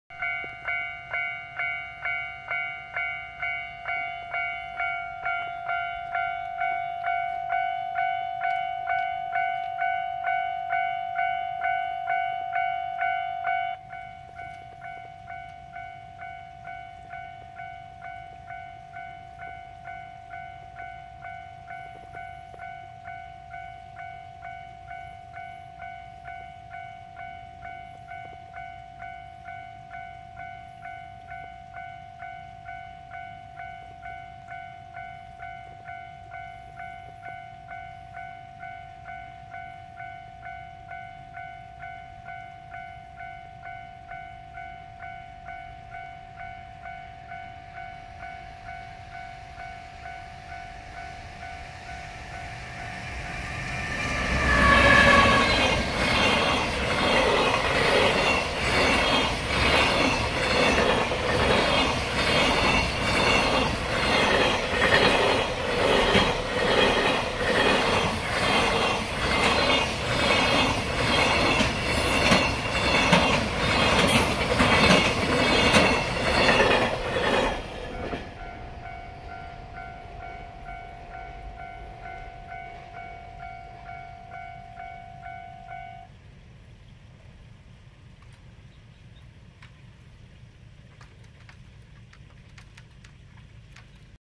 山陽本線 岩国市神代駅近くの踏切で 貨物列車上り岩国方面を駆け抜ける。
山口県岩国市神代（神代駅）の
岩国寄りの踏切。
踏切前は瀬戸内海、国道188号線が
あります。